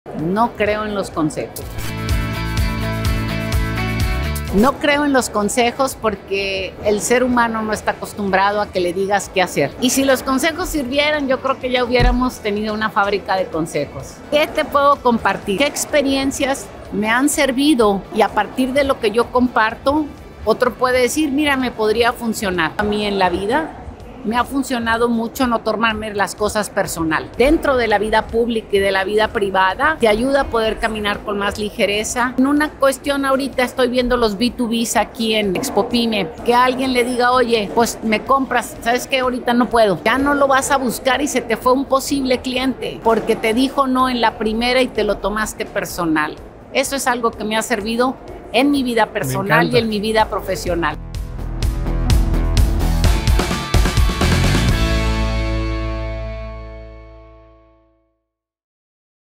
PodcastTransforma | Tatiana Clouthier
El PodcastTransforma, ¡Próximamente! Con la invitada especial: Tatiana Clouthier, Ex-Secretaria de Economía de México.